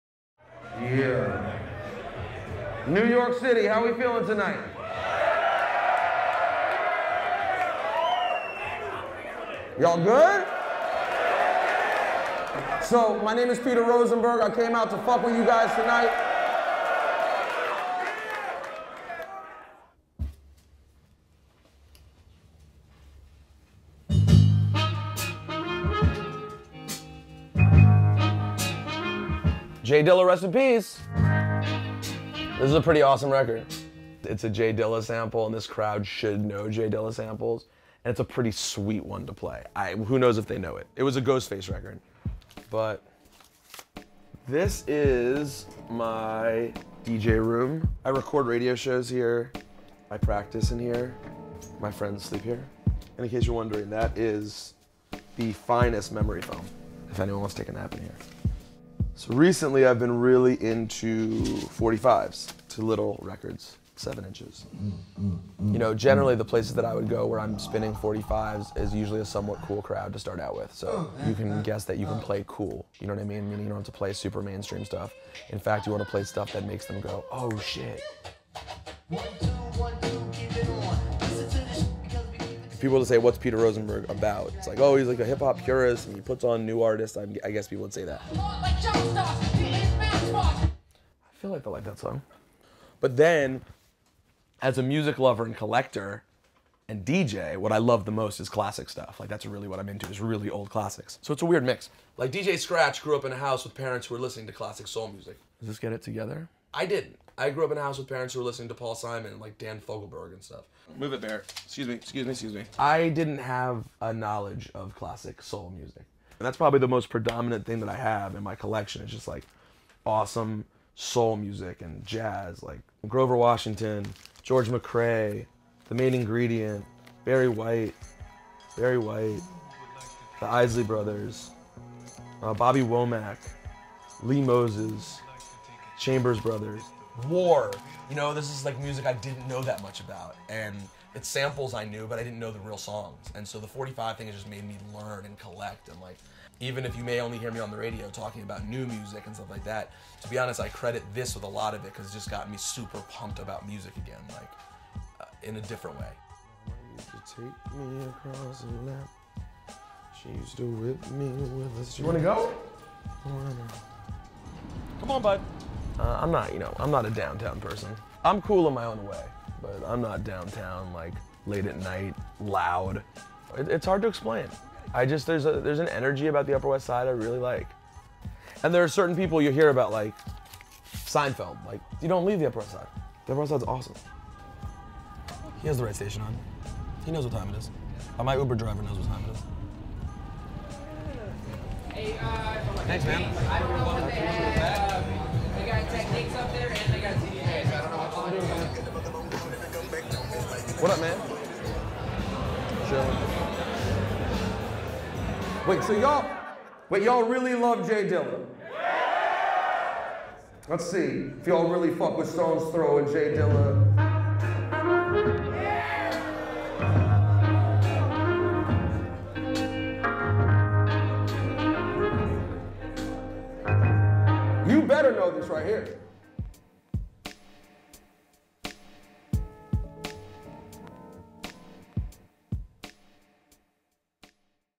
Peter Rosenberg: A DJ Who Actually Plays Records
Peter Rosenberg is a rap purist and a d.j. on the radio station Hot 97. These days, many d.j.s don't actually d.j.—some play sets straight from iTunes; others act more like record producers, or commentators—but Rosenberg still has a thing for vinyl.